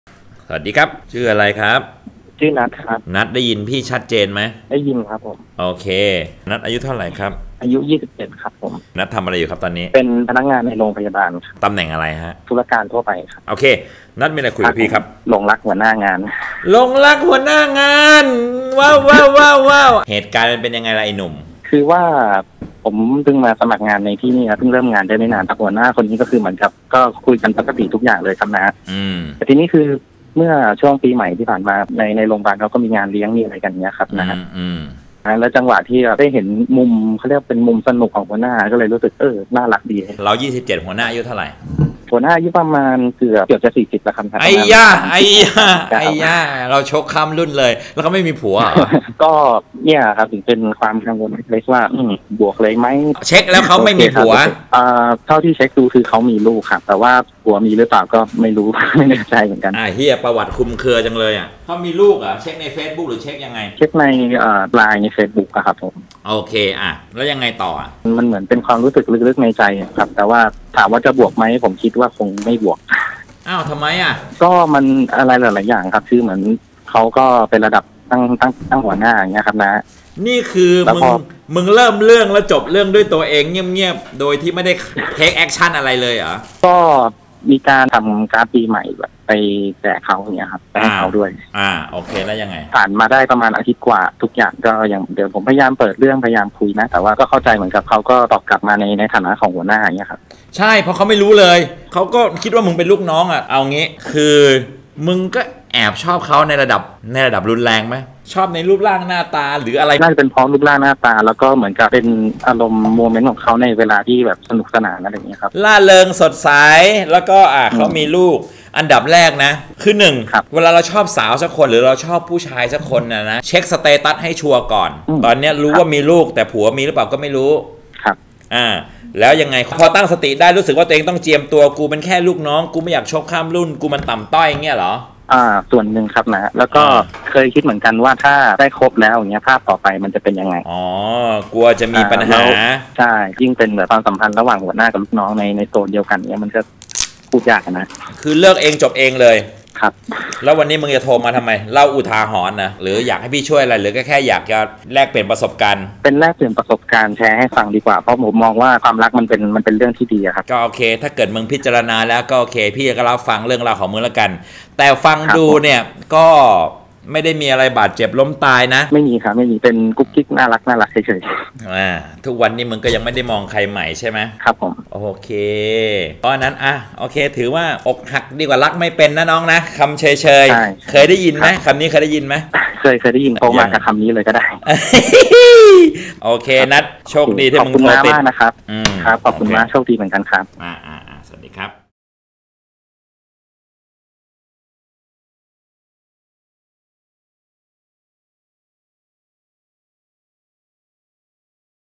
อย่าหาว่าน้าสอน หลงรักหัวหน้า....ไม่กล้าหรือเจียมตัว???
ความรักเกิดขึ้นได้ทุกที่ และกับทุกคน..

ไม่ว่าคนนั้นจะเป็นใครอายุเยอะกว่าหรือตำแหน่งสูงกว่าก็ตามแต่..

ลองมาฟังเรื่องของหนุ่มคนนึงที่หลงรักหัวหน้างานกัน !!!